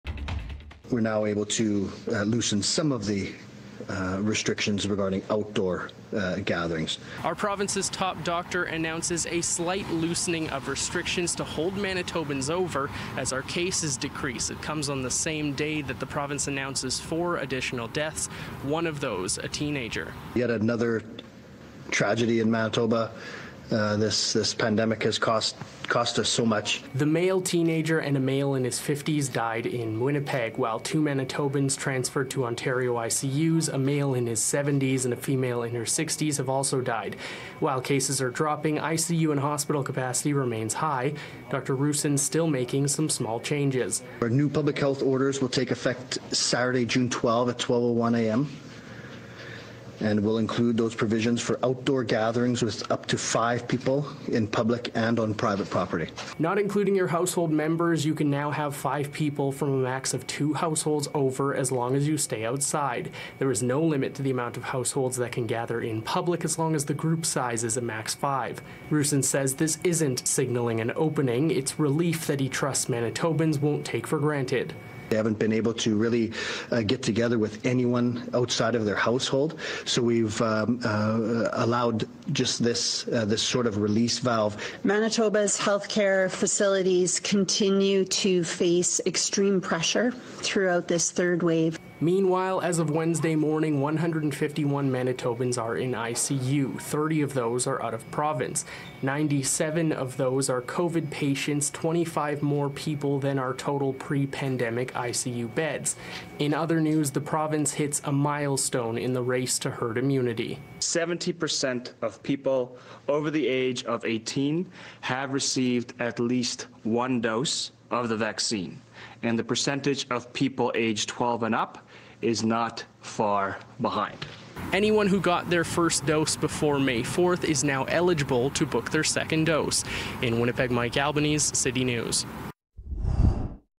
Manitoba restrictions begin to ease, doesn’t signal reopening
Manitobans will now be able to invite up to 5 guests from a maximum of 2 different households to their property or meet in a group of 5 people max on public property with new changes to public health restrictions.